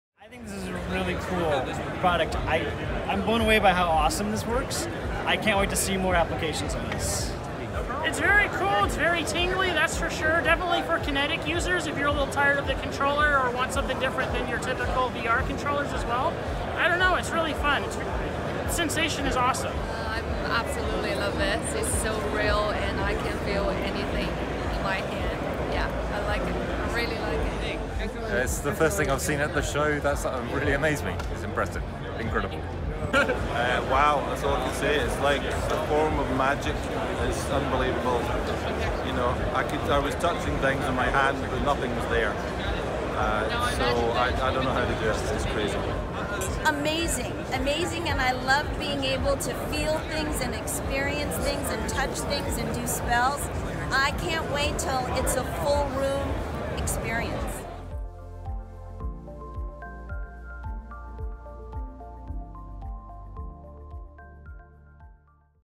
GDC 2018: Reactions to Ultrahaptics technology
From VR magic to interactive posters, hear what the crowd at the Games Developer Conference 2018 had to say on Ultrahaptics technology.